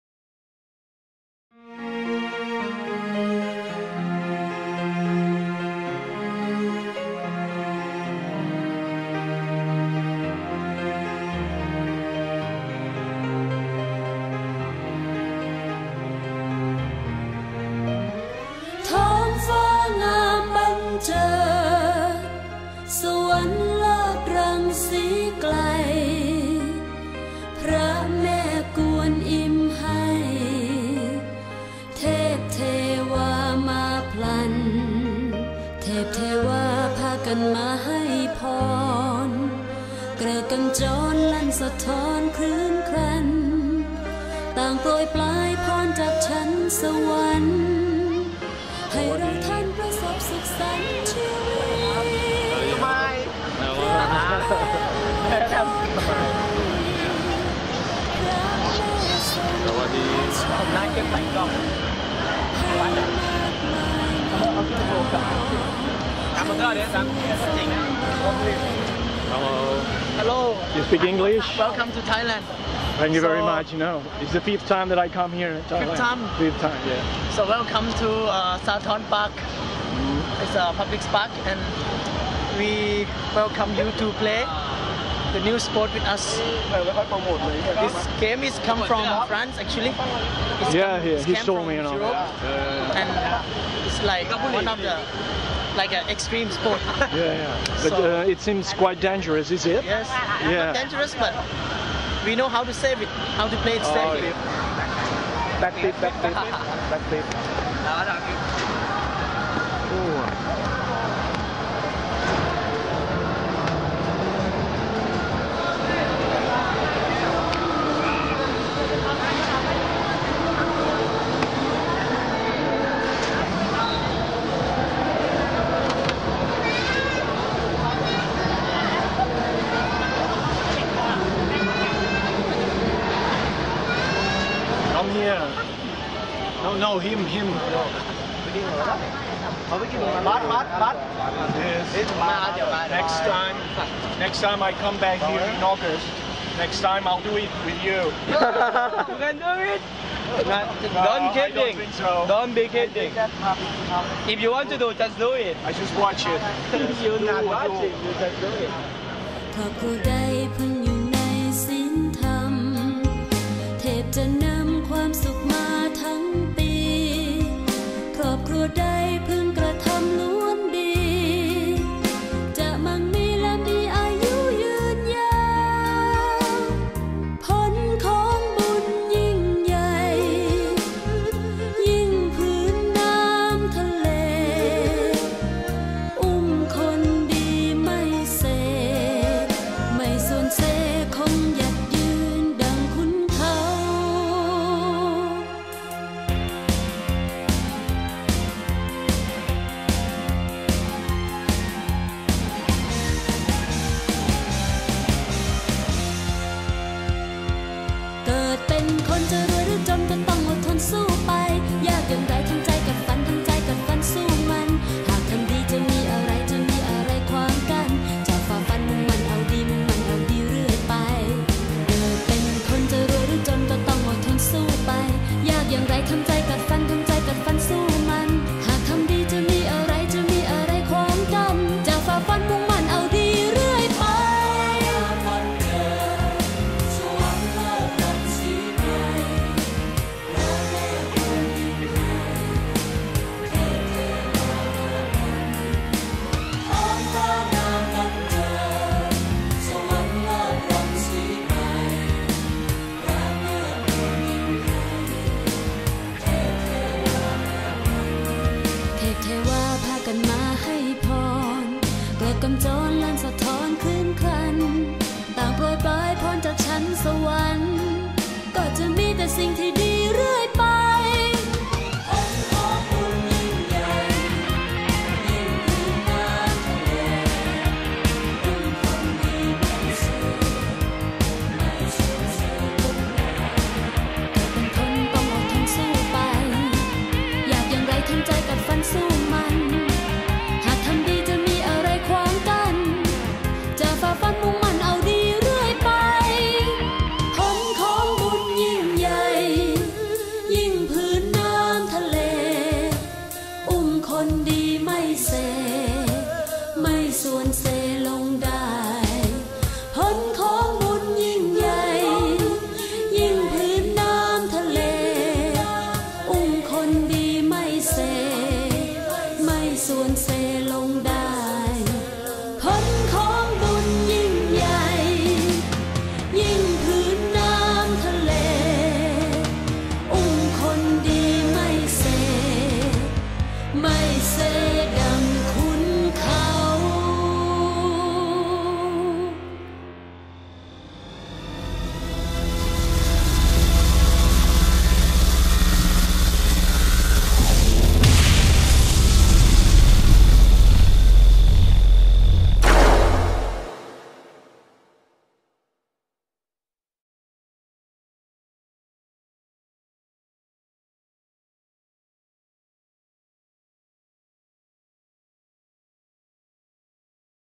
06 Shirtless boys practice Parkour at Sathorn Park - Bangkok, Thailand (Clip)
Quim meets a group of Thai boys who show him how to play Parkour at Sathorn Park near Chao Pra Ya river. These young cute and nice boys also used to practice Free running across the park, even though they wouldn't do it that day. What's shown in the clip isn't actually a professional performance, but a minor demonstration of what the older teen boys were teaching to the younger ones. "The youngest boys come to see what it feels like to play this game; they want to try it, but it's hard. Some of them stay, and the rest don't come anymore. There are new faces every month..." said the most experienced boy of the group.
I want to thank them all for their kindness and friendliness.
The video was shot in March 2010 -- Re-edit in June 2014
Saturn Produccions JC -- Cassà de la Selva.
Inclou subtítols en català.
Un grup de nois adolescents ensenyen a en Quim com s'entrenen en un joc d'habilitat física anomenat Parkour.
กีฬาสุดยอดมันส์ ขอบคุณเด็กผู้ชายทุกคนที่เล่น PARKOUR ที่สวนสาทร กรุงเทพฯ เมืองไทย แม่น้ำเจ้าพระยา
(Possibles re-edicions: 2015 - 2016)